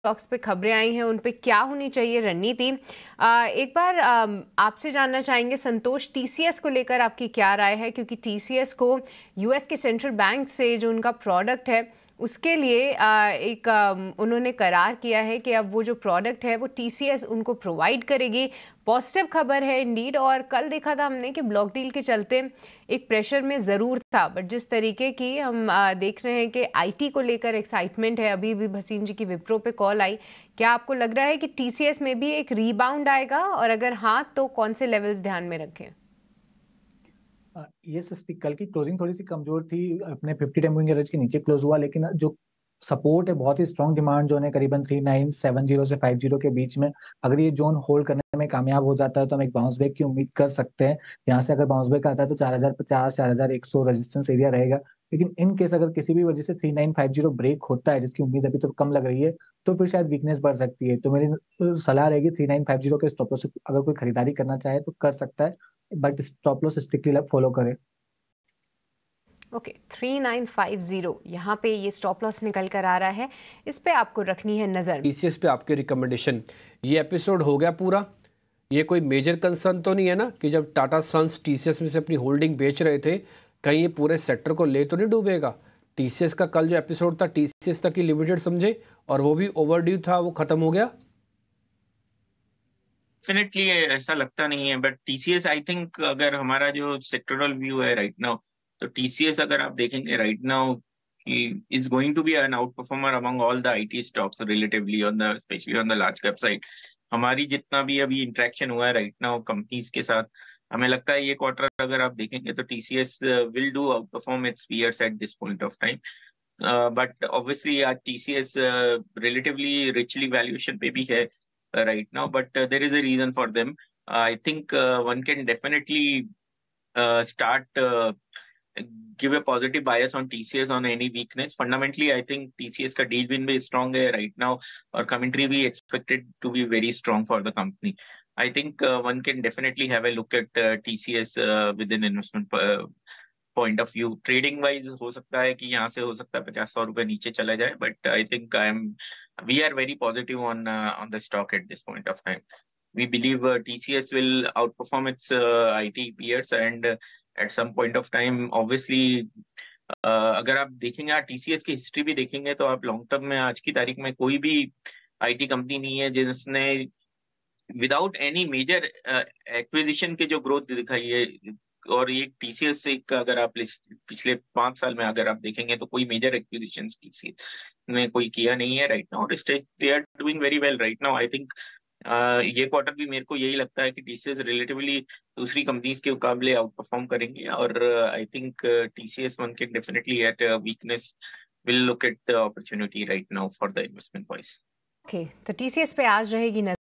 TCS Share में Block Deal के चलते उथल-पुथल, Experts से जानिए Share में बन रहे हैं खरीदारी के मौके?
TCS Share Price, TCS Share Buy, Sell or Hold Recommendation , TCS Share Price Analysis, TCS Stock Target & Stop Loss , ET Now Swadesh ,
TCS Share में जानें निवेश को लेकर क्या है Experts की राय. जानें किन levels पर करनी चाहिए आपको खरीदारी और क्या है Target Price और जानिए किस Strategy के साथ इस Stock में पैसा कमाया जा सकता है. देखें वीडियो.